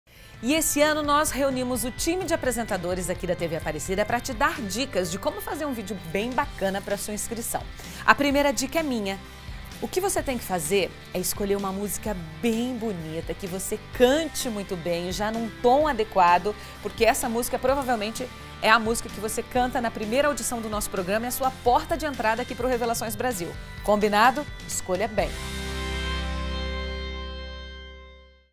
Amanda Françozo dá dicas para se inscrever no Revelações Brasil| Inscrições já estão abertas!
O Revelações agora é Brasil e você já pode se inscrever para participar do reality musical da TV Aparecida. Nesta edição, os apresentadores da TV Aparecida vão te dar algumas dicas para você fazer a sua inscrição. Confira a fica da apresentadora Amanda Françozo!

TV APARECIDA
É bom estar onde a Mãe está!